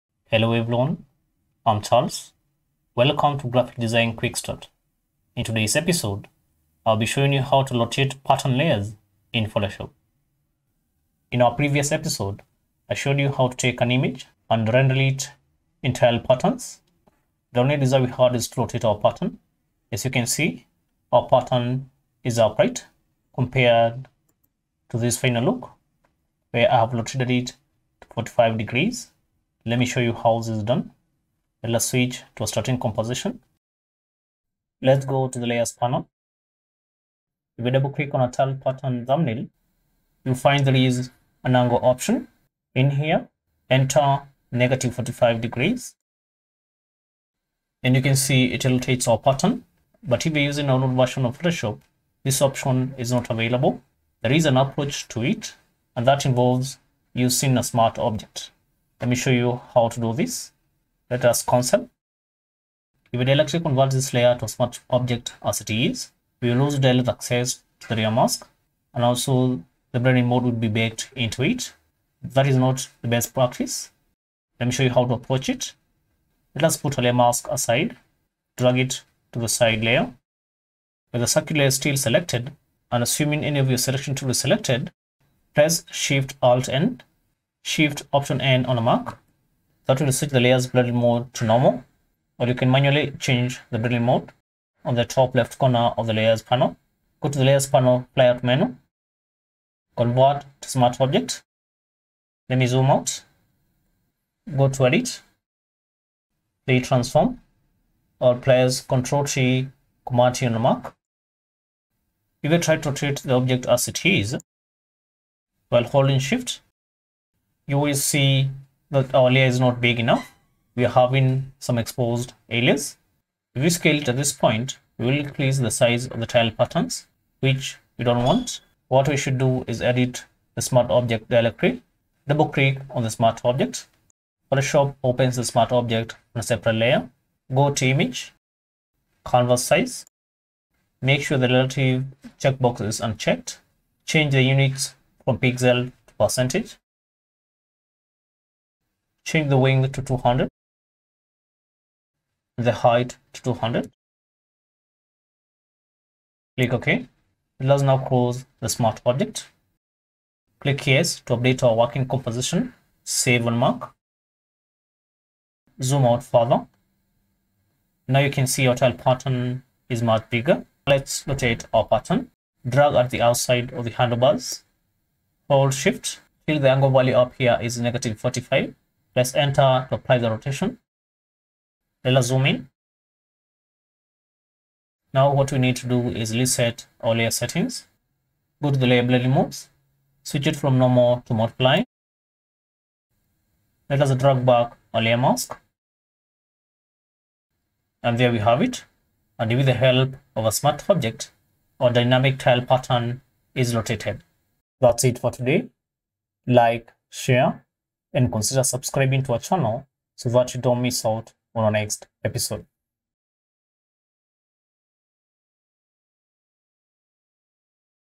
44-Rotating a pattern layer in photoshop
Rotating Pattern Layers in Photoshop is one of the coolest new features found in the June 2020 release of Photoshop. However, if you are using an older version on photoshop this option is not available. In this tutorial, I take you through a step-by-step work around for rotating a pattern fill layer.

This feature in photoshop 2020 and later allows you to rotate patterns (non-destructively) to change the angle of the pattern. You can change the orientation of any pattern in Pattern Overlays, Pattern Strokes, and Pattern Fill Layers.

✍✍Make a point of practicing with your own or same images I've used✍✍